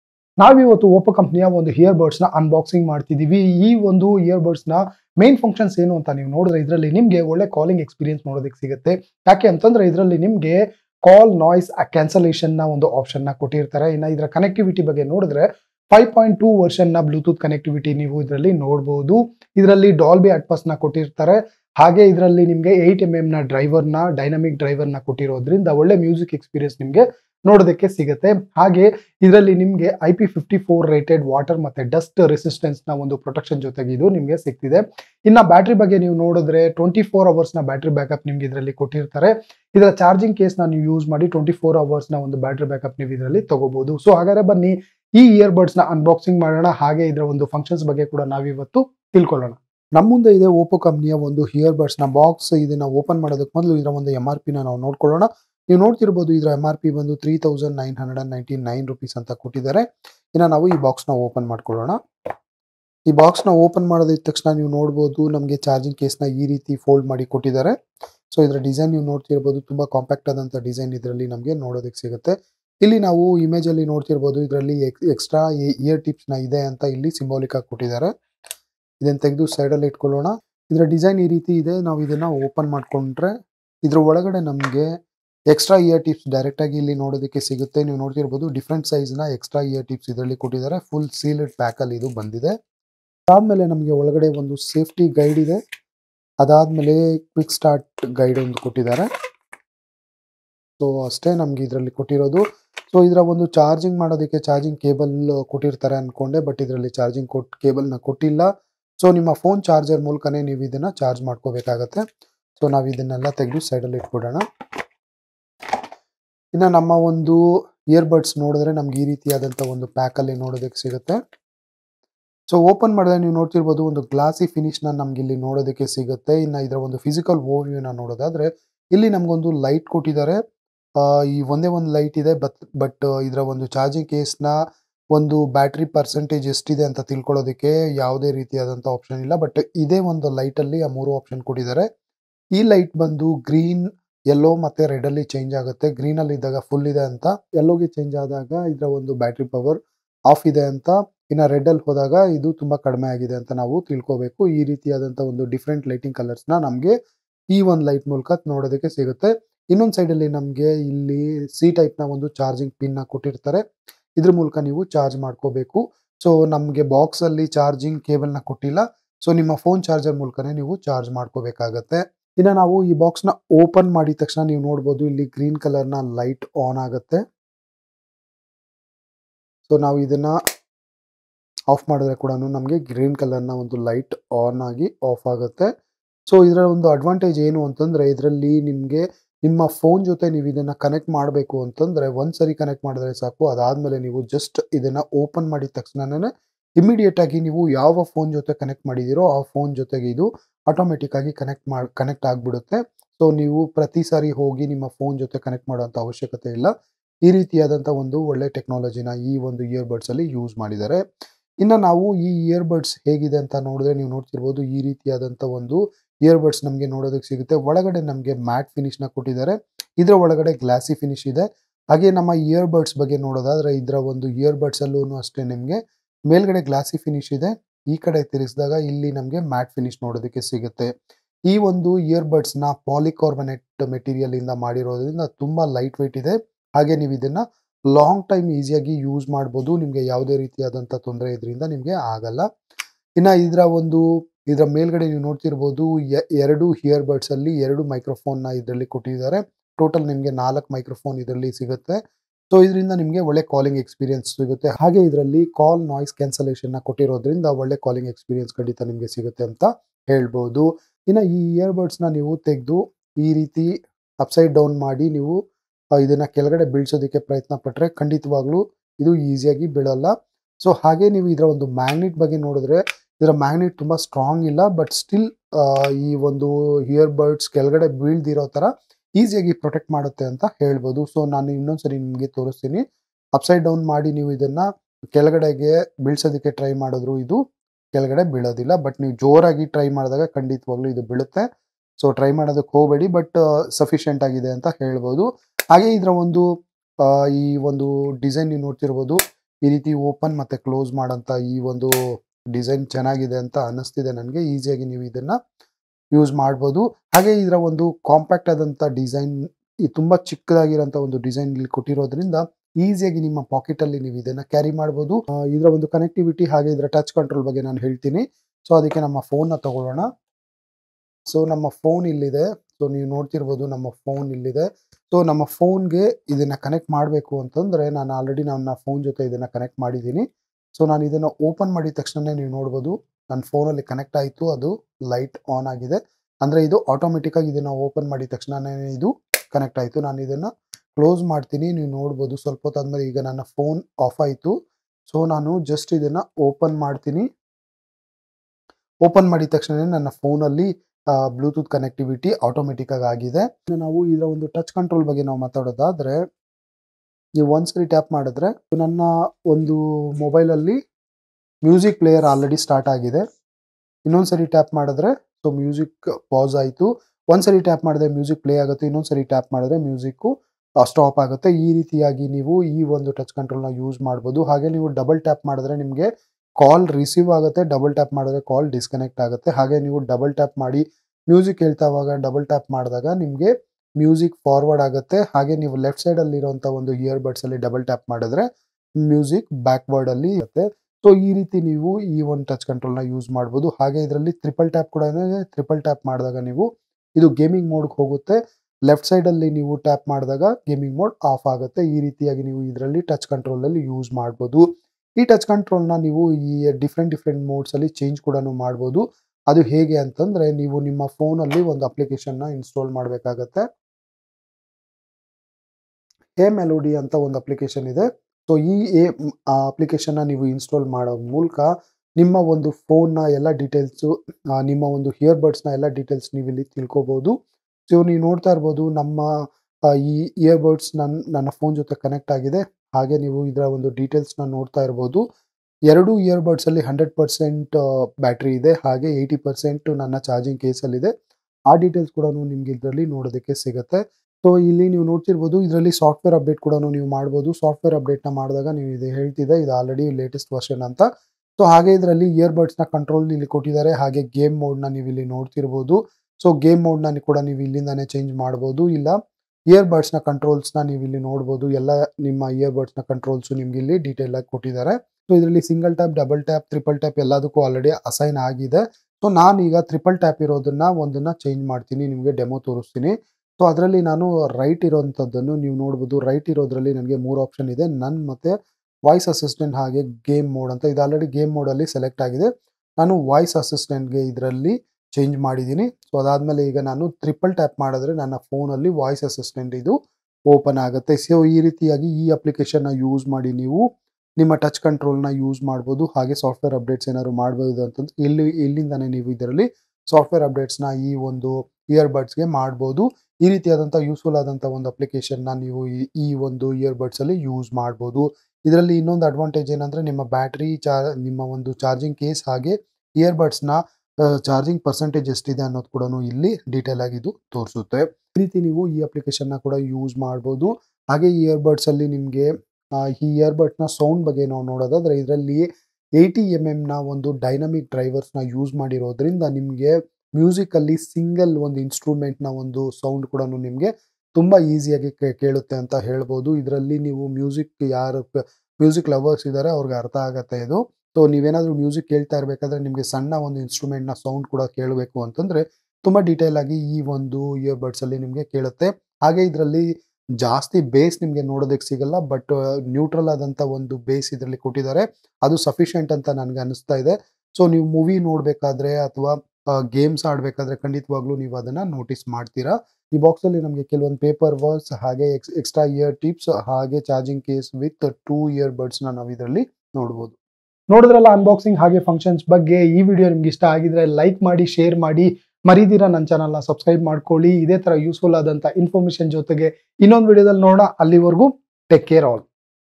OPPO Enco Buds || Budget Earbuds
With Mic: Yes
With 24 Hours Music On: 6H single-charge | 24H With Charging Case
Concert Like Experience With Great Bass:Equipped with TPU+PEEK material double-layer composite diaphragm
Clear Calls: Supports Intelligent Call Noise Cancellation
Quick Pair, Stable Connection: Bluetooth 5.2+, Atenna Array Optimization
80ms low-latency game mode: Perfect audio & video sync during the game
Support Dolby Atmos | IP54 Dust and Water Resistance.